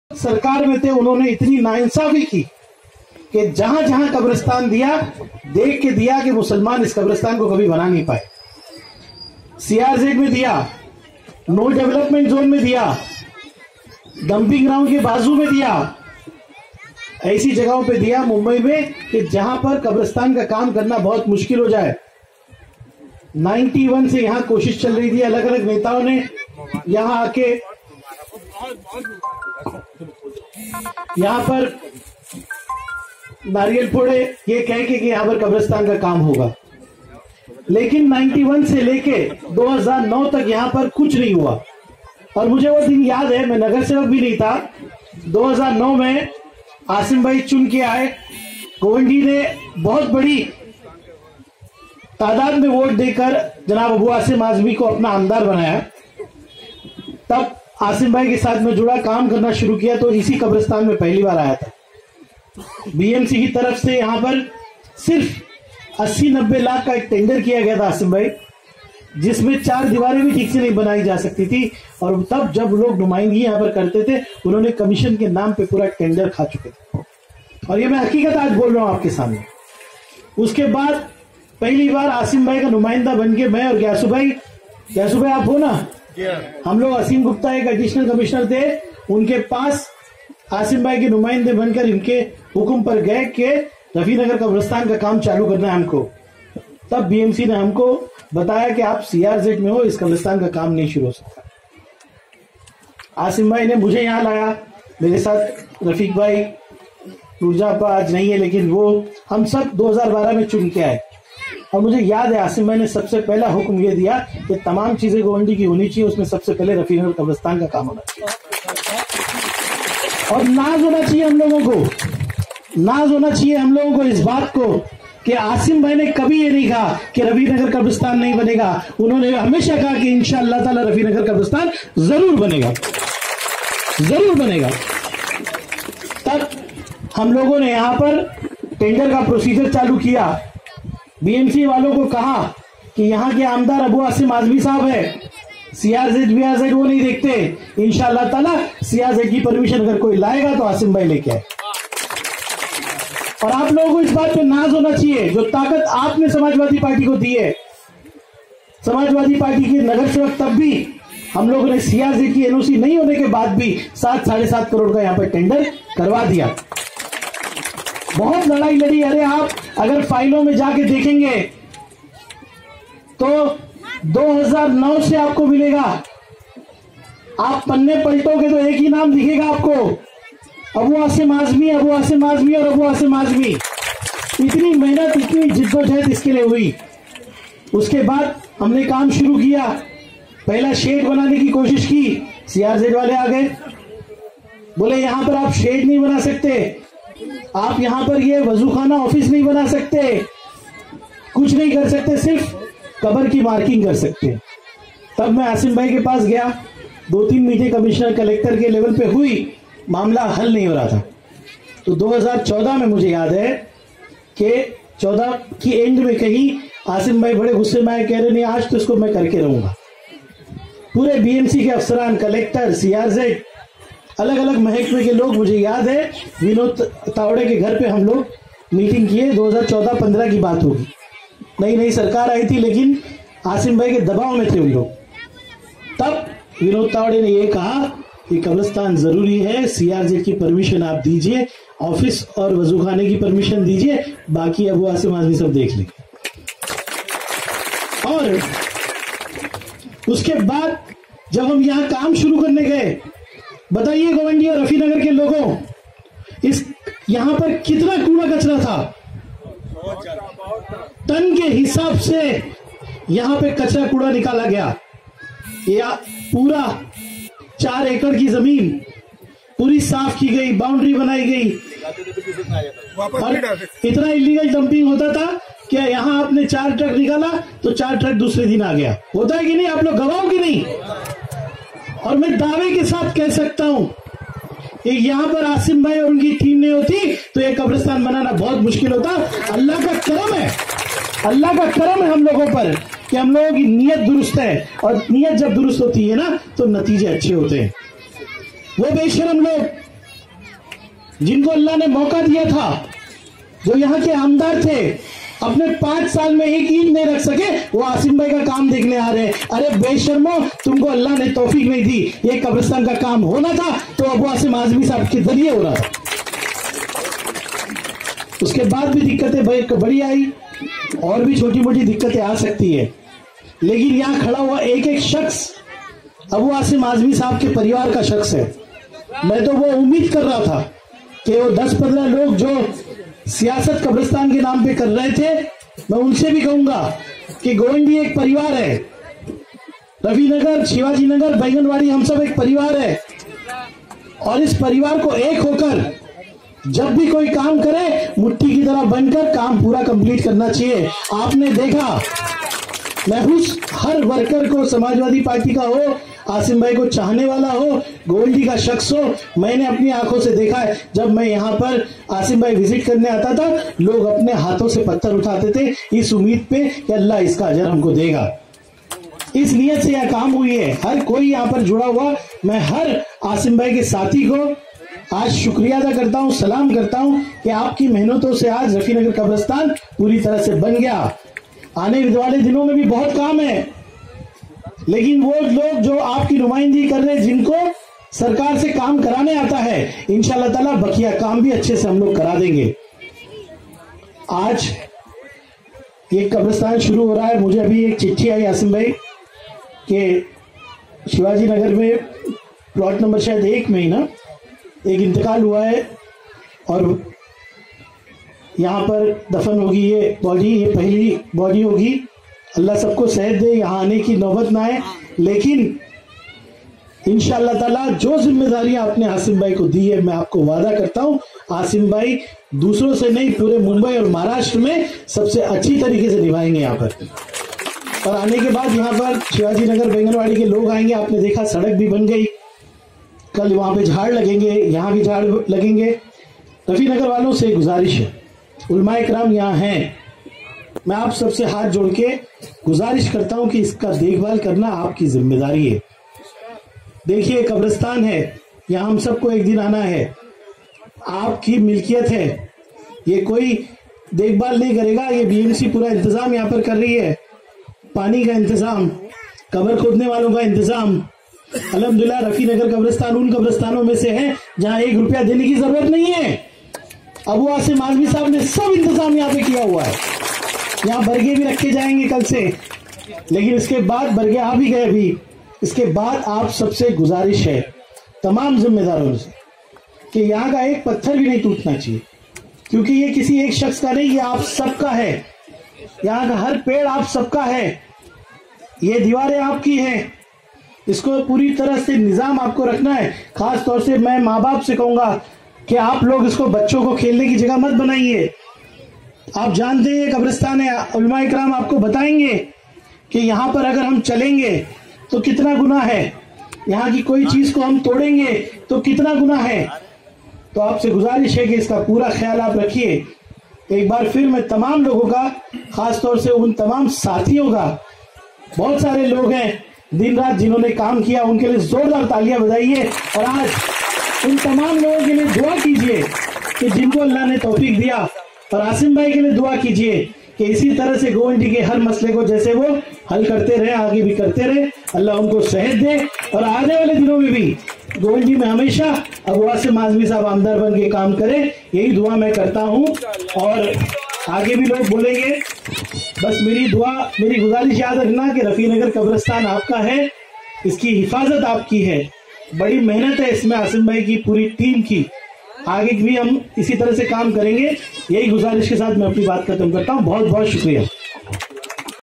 Haqeeqat | Kyun Nahin Poore Horahe Hain Maharashtra Mein Allotted Kabristan ke Kaam
Video from Saqib